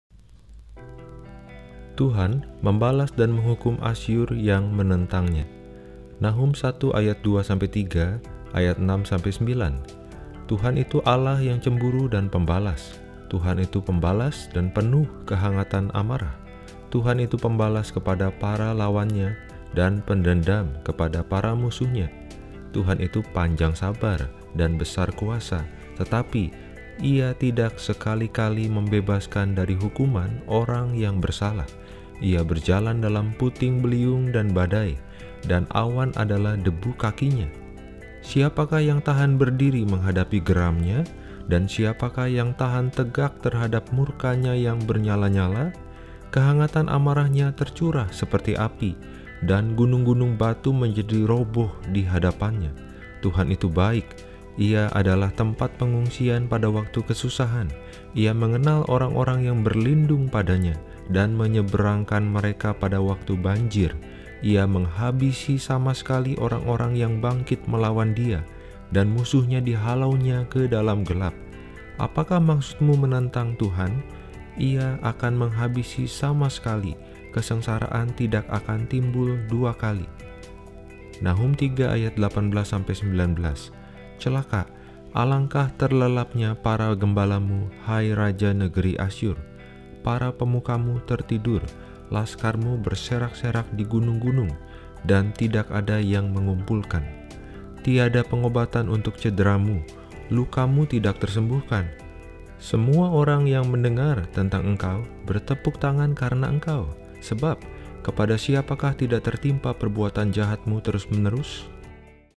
259 September 15 - TUHAN membalas Asyur yang menentang-Nya - Alkitab Satu Menit Sepanjang Tahun
Nahum 1:2-3, 6-9
1:2 TUHAN itu Allah yang cemburu dan pembalas, TUHAN itu pembalas dan penuh kehangatan amarah. TUHAN itu pembalas kepada para lawan-Nya dan pendendam kepada para musuh-Nya.
1:3 TUHAN itu panjang sabar dan besar kuasa, tetapi Ia tidak sekali-kali membebaskan dari hukuman orang yang bersalah. Ia berjalan dalam puting beliung dan badai, dan awan adalah debu kaki-Nya.
1:6 Siapakah yang tahan berdiri menghadapi geram-Nya? Dan siapakah yang tahan tegak terhadap murka-Nya yang bernyala-nyala? Kehangatan amarah-Nya tercurah seperti api, dan gunung-gunung batu menjadi roboh di hadapan-Nya.
1:7 TUHAN itu baik; Ia adalah tempat pengungsian pada waktu kesusahan; Ia mengenal orang-orang yang berlindung kepada-Nya
1:8 dan menyeberangkan mereka pada waktu banjir. Ia menghabisi sama sekali orang-orang yang bangkit melawan Dia, dan musuh-Nya dihalau-Nya ke dalam gelap.
1:9 Apakah maksudmu menentang TUHAN? Ia akan menghabisi sama sekali; kesengsaraan tidak akan timbul dua kali!

Nahum 3:18-19
3:18 Celaka! Alangkah terlelapnya para gembalamu, hai raja negeri Asyur! Para pemukamu tertidur, laskarmu berserak-serak di gunung-gunung, dan tidak ada yang mengumpulkan.
3:19 Tiada pengobatan untuk cederamu, lukamu tidak tersembuhkan. Semua orang yang mendengar tentang engkau bertepuk tangan karena engkau; sebab kepada siapakah tidak tertimpa perbuatan jahatmu terus-menerus?